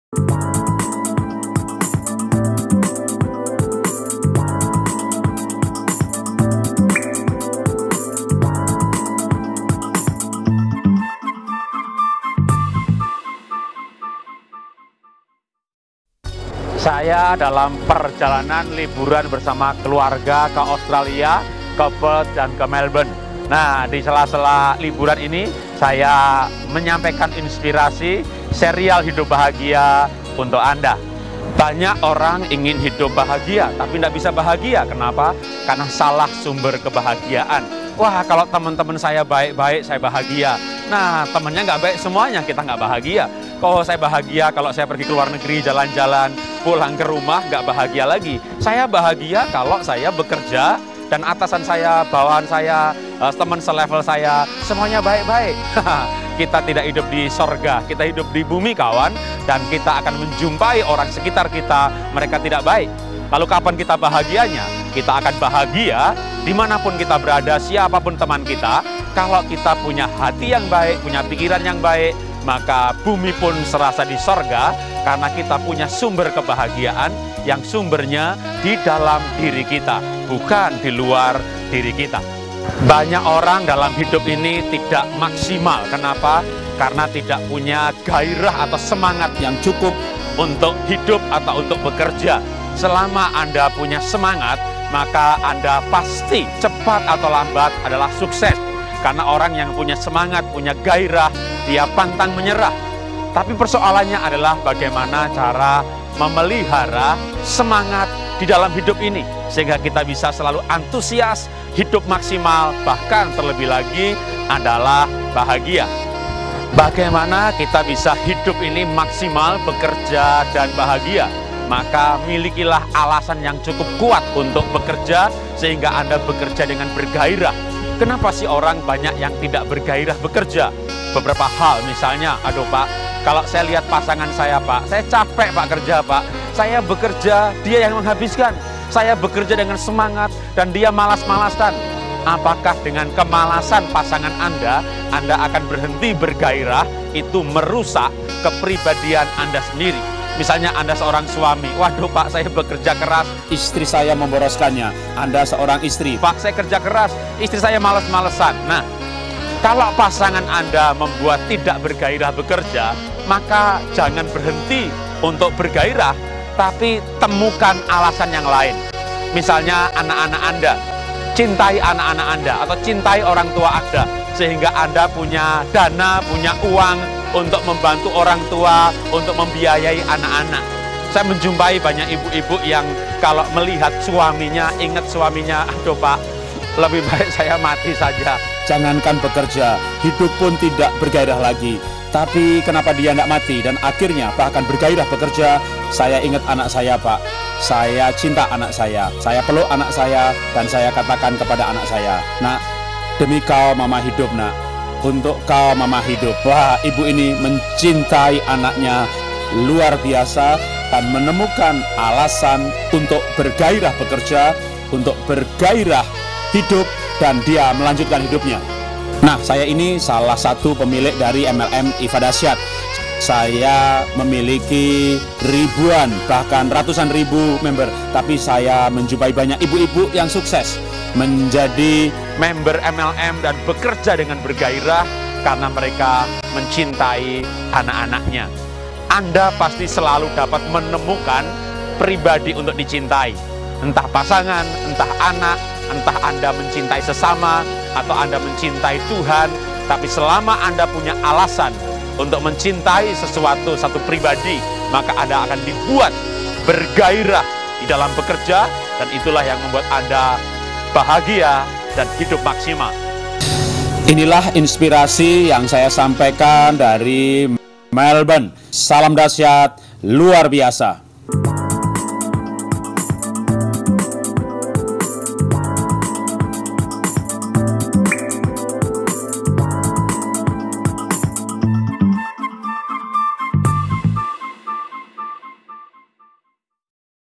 Inspirasi Hidup Bahagia 14, Melbourne, Ir. Jarot Wijanarko
HIDUP BAHAGIA? Inspirasi Melbourne 14. Pertama bahagia kalau kita memiliki hati yang baik, pikiran yang baik dan itu membuat kita bahagia karena memiliki sumber kebahagiaan yang ada di dalam hati kita, yang mengalir keluar mempengaruhi orang2 disekitar kita, dan jangan sebaliknya, kita dipengaruhi lingkungan yang jahat, teman yang acuh, usaha yang penuh masalah dan membuat stress. Kedua, temukan alasan hidup, alasan bekerja yang membuat anda bergairah bekerja.  Alasan hidup yang mulia membuat orang hidupnya kuat. Simak inspirasi selengkapnya, dalam video streaming ini.
Masih banyak episode lainya di u-tube, ‘Inspirasi Jarot Wijanarko’.